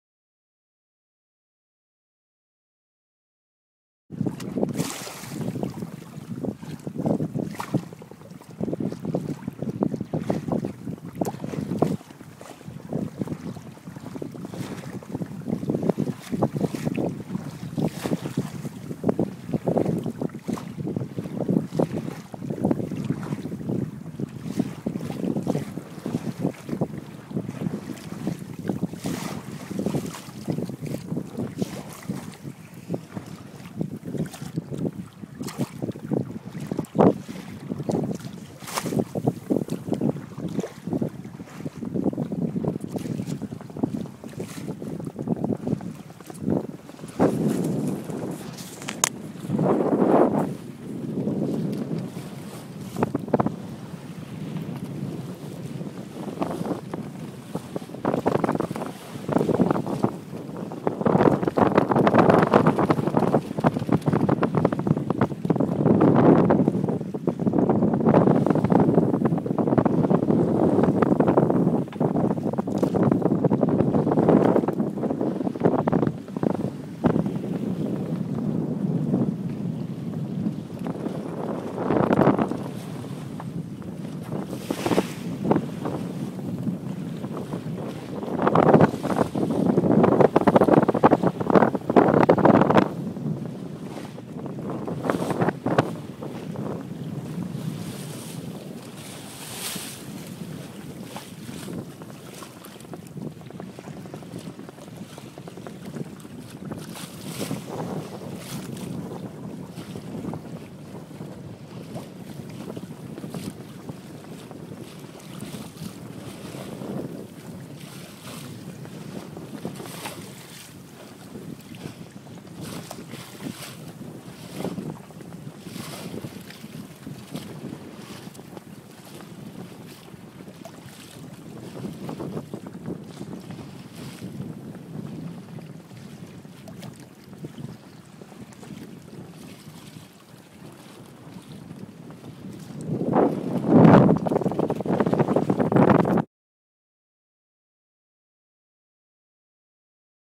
Sailing on the 20 ft Adria on Lake Ontario on 30 JUN 2012
This is not much of a movie but rather a record to jog memory in future about a very nice excursion on Lake Ontario. Sue at the helm and Molly asleep below. This 2.5 minute video clip shows only the boat plying through 2 foot waves and +/- 10 knot winds on the still cold (72 degrees F) waters of the lake. The boat is a Heritage class built by Howie Renner in Portalnd, OR in 1967.